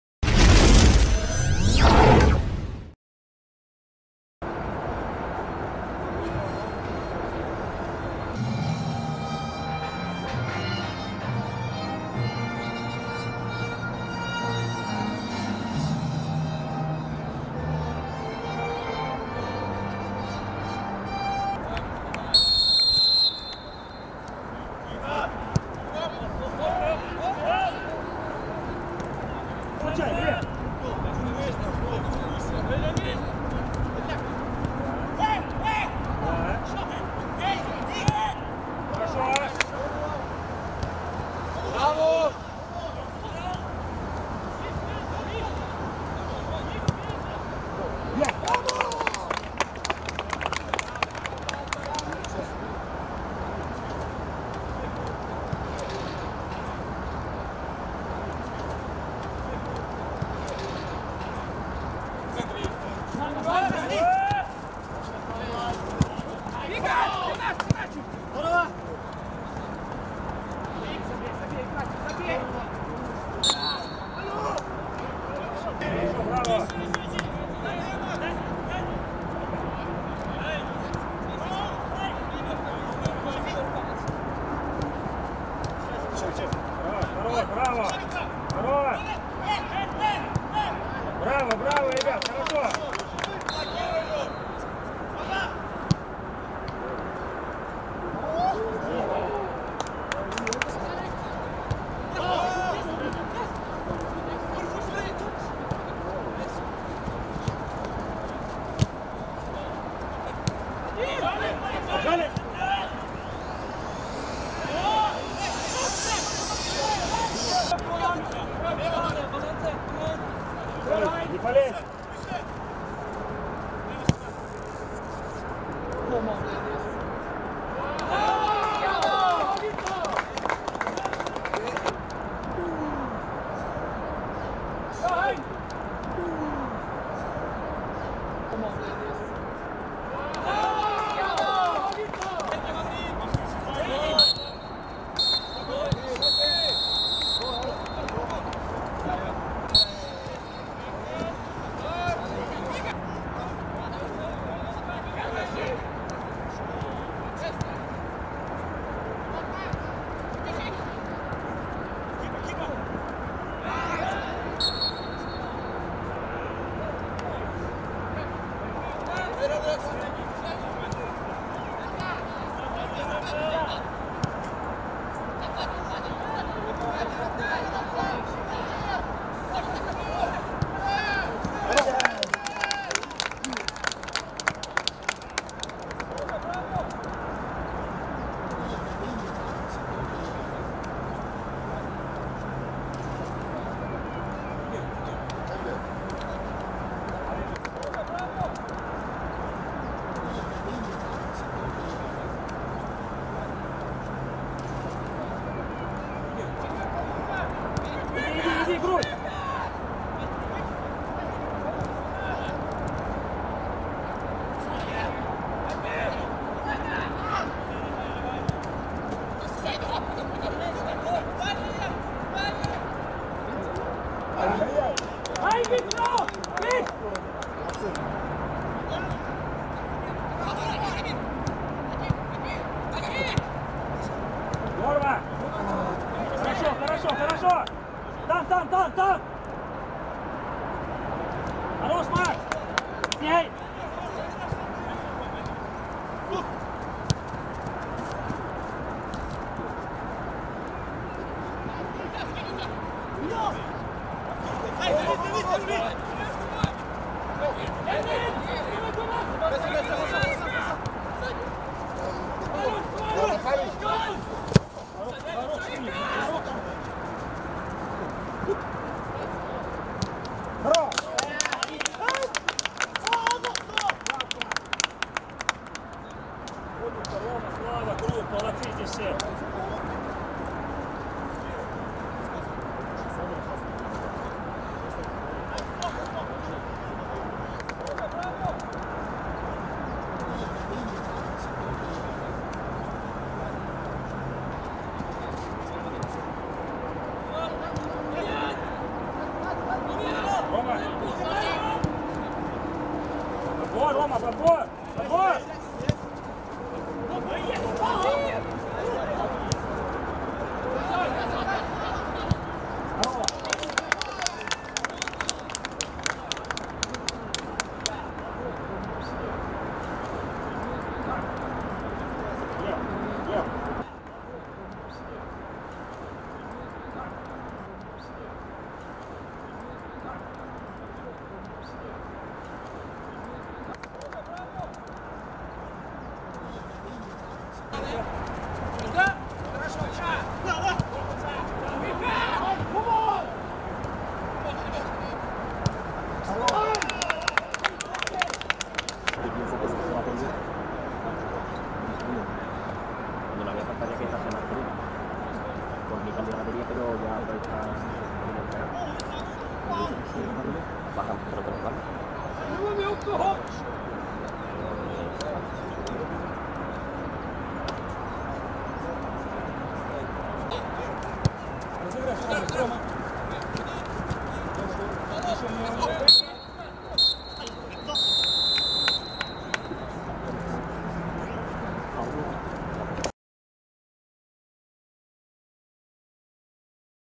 Россия - Норвегия - 5:1. Молодёжные сборные. Контрольный матч. Обзор матча | РФС ТВ
В понедельник, 25 марта, молодежная сборная России нового созыва (игроки не старше 1998 года рождения) провела второй контрольный матч на подходящем к концу сборе в испанской Марбелье. Наша команда под руководством Михаила Галактионова одержала крупную победу над Норвегией со счетом 5:1.